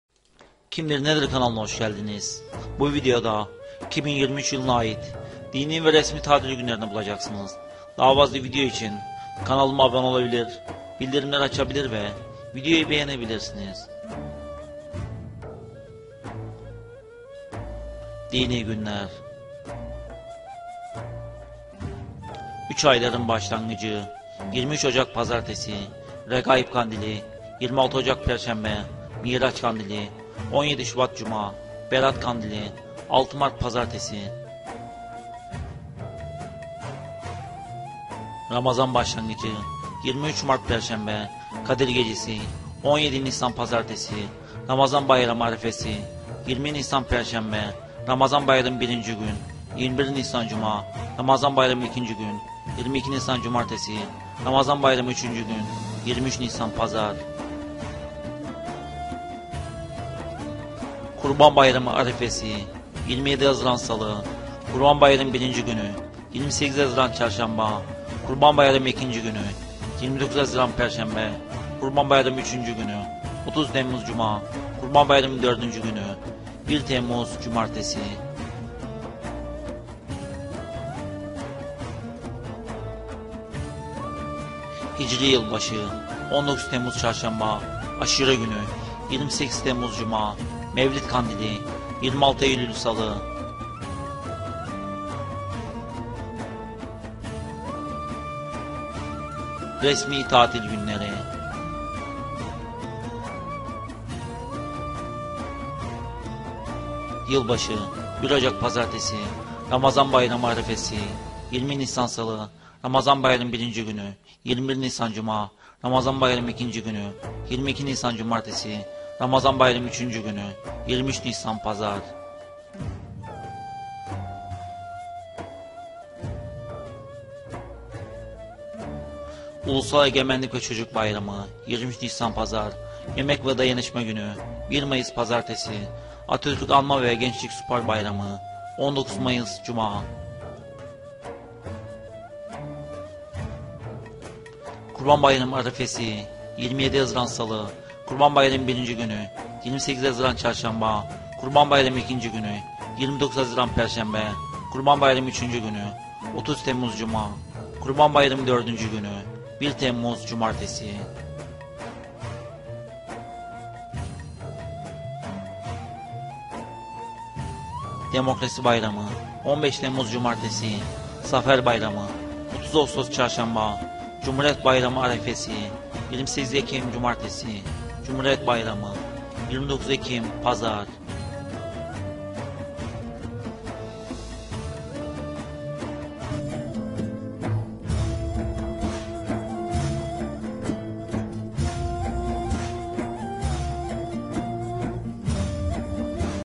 2023 Yılına Ait Dini ve Resmi Tatil Günleri
Bu videoda 2023 Yılına Ait Dini ve Resmi Tatil Günlerini bulacaksınız.
Diyanet.